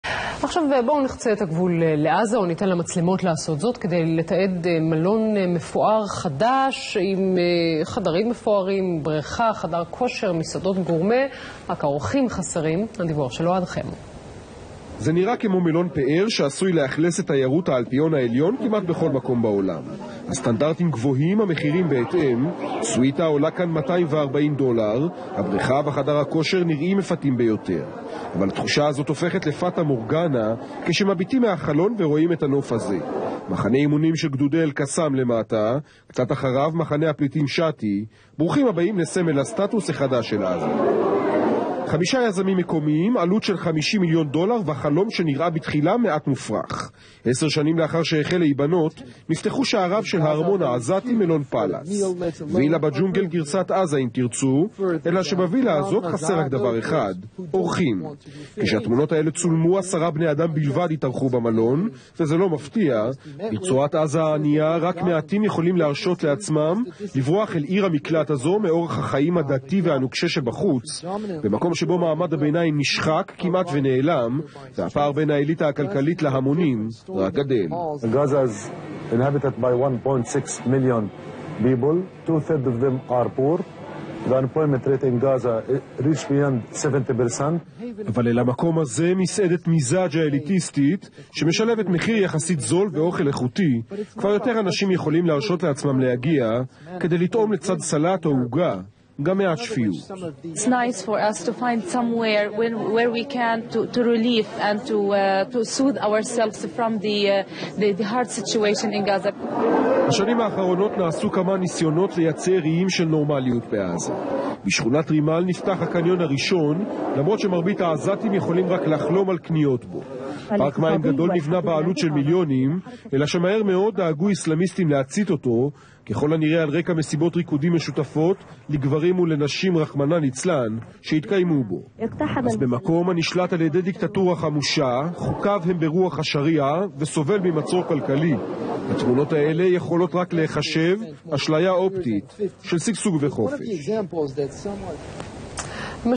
הכירו: מלון 5 כוכבים בעזה
סוויטות במאות דולרים ללילה, חדר כושר, בריכה גדולה ומסעדות גורמה - הכל במלון חדש, קרוב מאוד לאשקלון, מלון שלישראלים לפחות אסור לבקר בו. עזה מזווית אחרת